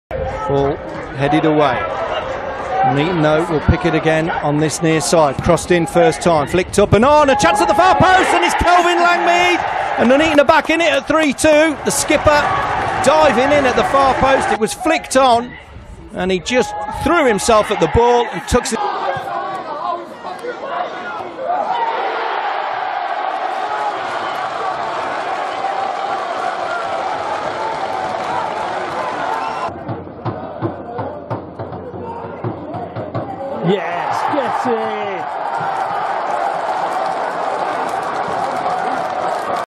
Kelvin Langmead 2016-17
Goals scored by Kelvin Langmead for Nuneaton Town during the 2016-17 National League North season.